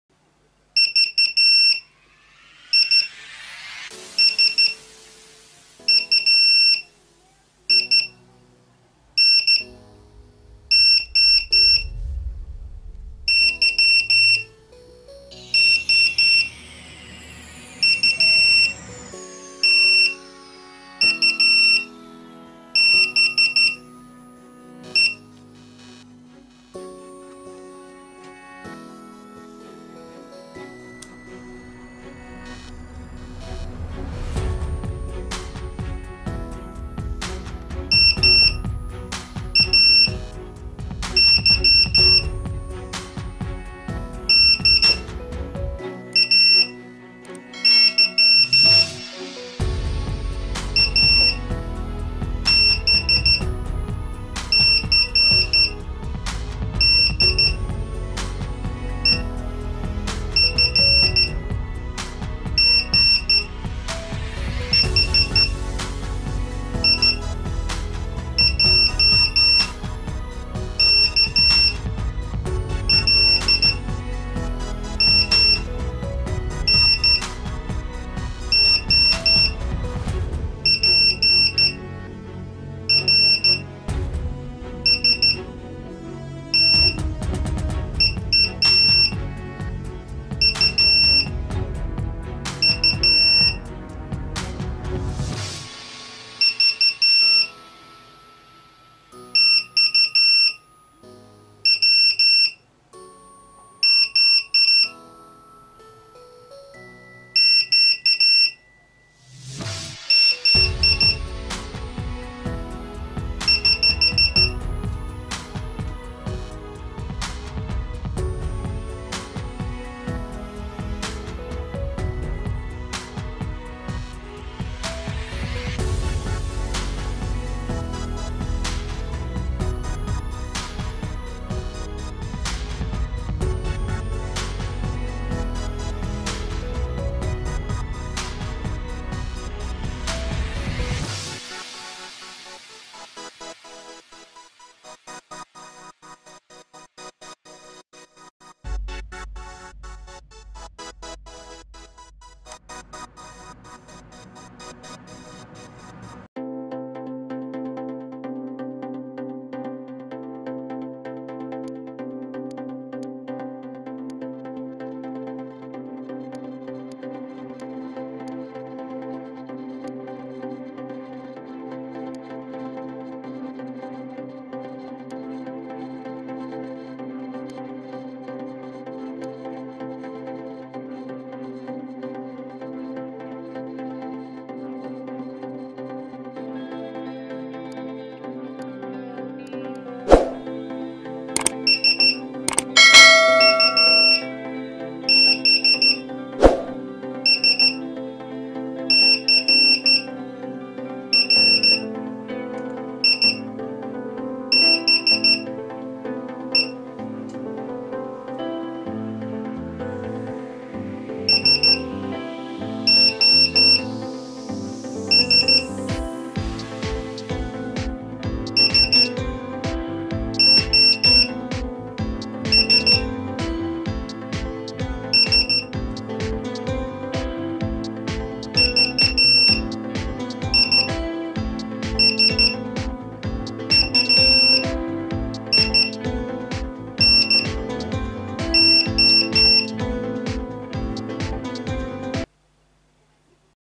Arduino Project-Morse Decoder And Transmitter
Free code!

How to make a morse code decoding machine with Arduino and Visuino. A project for beginners with wireless transmission of the alphabet to another device.
Arduino simple project. Morse alphabet transmitter and receiver with nrf24L01 module.
Amazing  NRF24l01 Aplication.